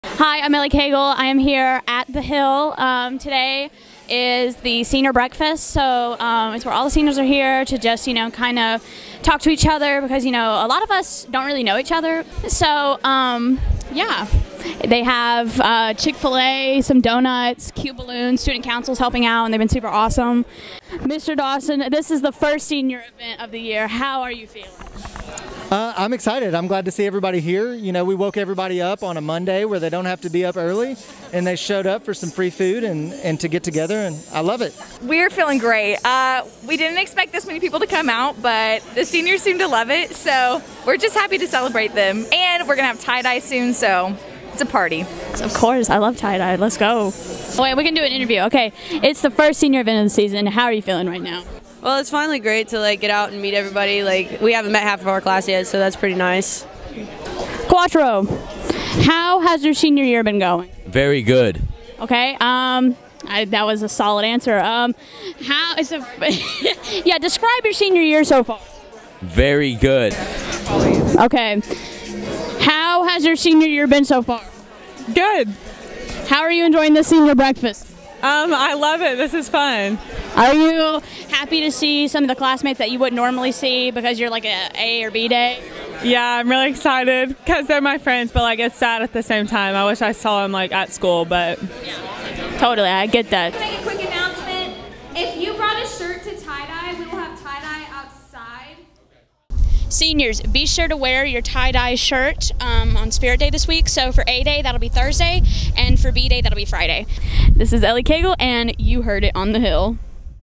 Our First Senior Breakfast!
Seniors gathered at Green Hill today for breakfast and to tie dye a t-shirt in our school colors. This event was organized by Student Council to kick off our Homecoming Week!